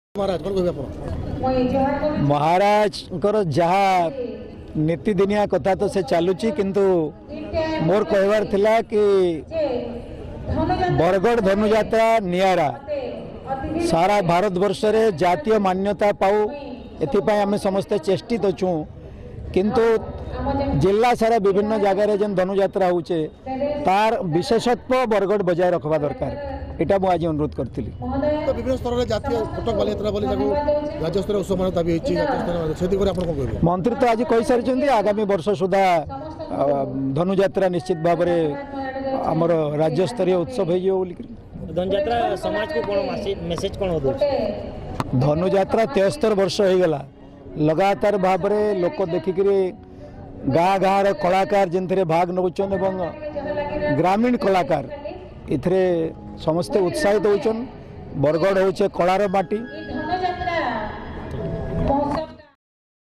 Bargarh dhanujatra 2020-guest Sri Subash chouhan -bargarh tv
World famous bargarh dhanujatra mahotsab 2020 . chief guest wodc chairman Sri Subash chouhan .